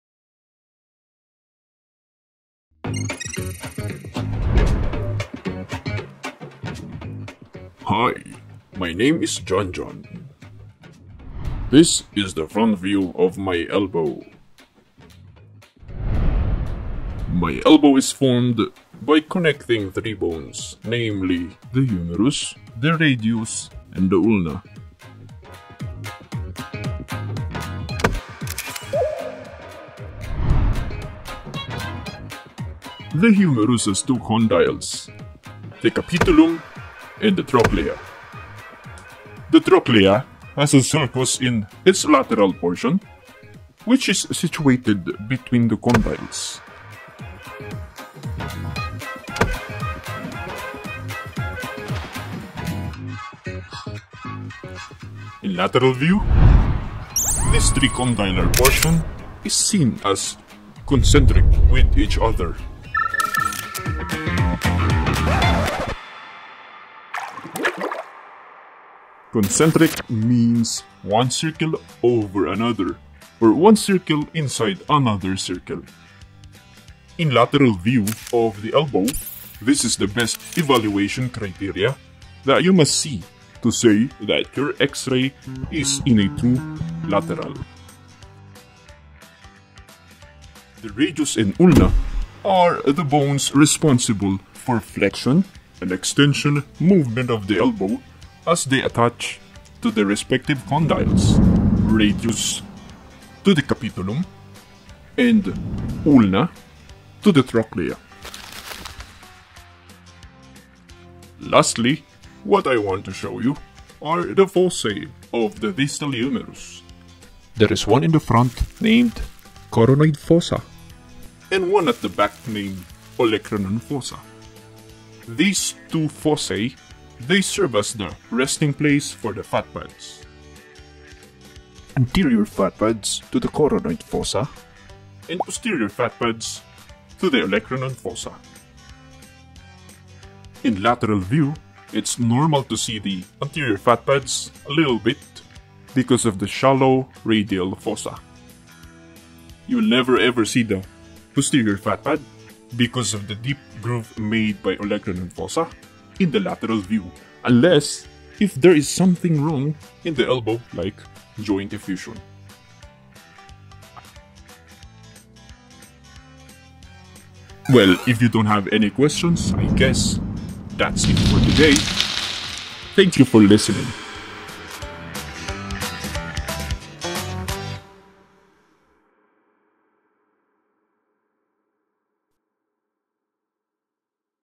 Anatomy of the elbow joint | Elbow radiography | X-ray of the elbow | Elbow positioning
My skeleton friend named "JanJan" will guide and help you understand the importance of knowing the anatomy of the elbow in relation to acquiring a true and a proper lateral radiographic image of the elbow joint.